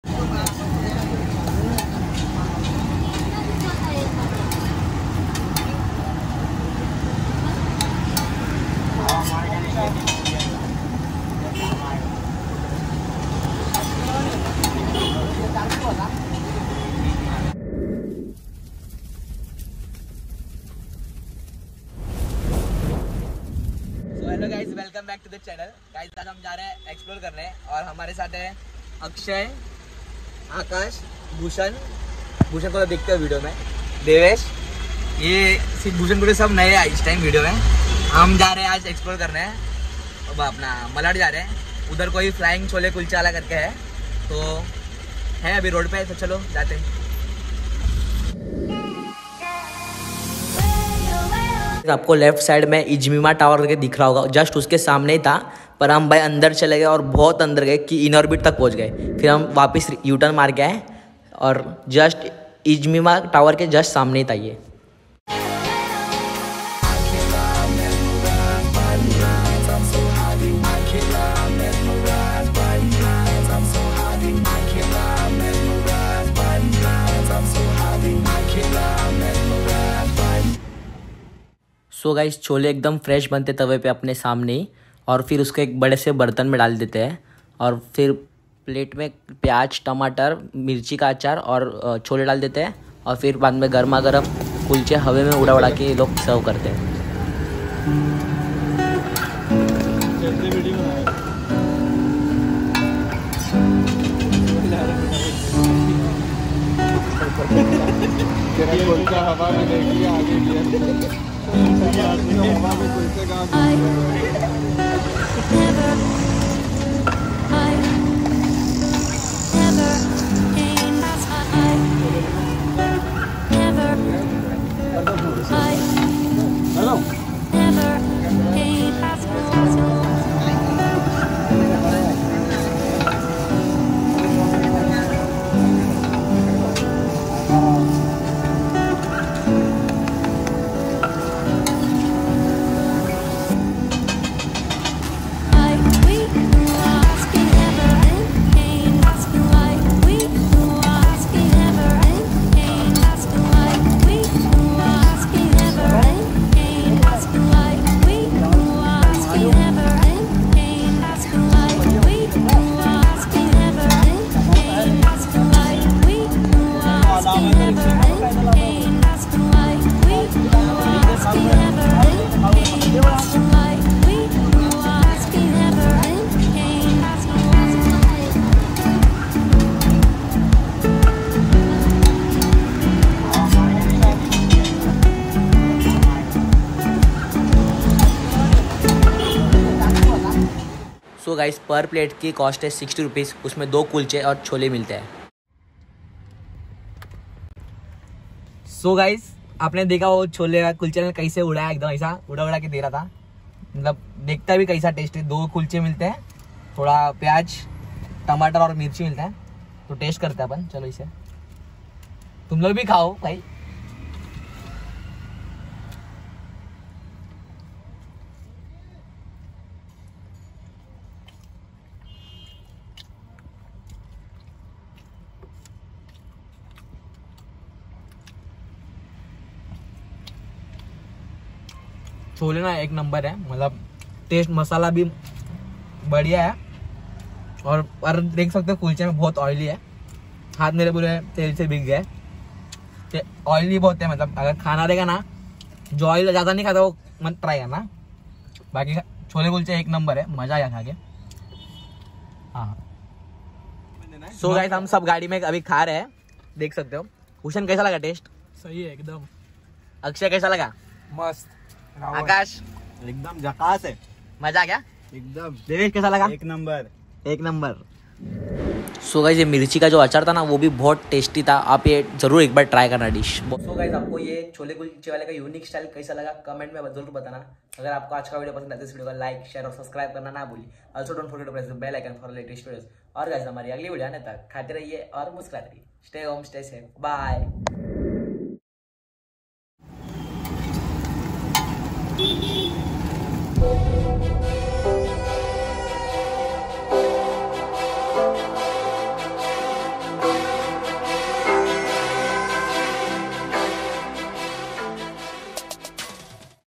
Flying Kulchawala of Mumbai | Unique Style | Indian Street Food
Address:- Infinity mall lane, near mindspace, ijmima tower, opposite interface, malad west
So guys in this video we explored chole kulchawala who serves flying kulcha.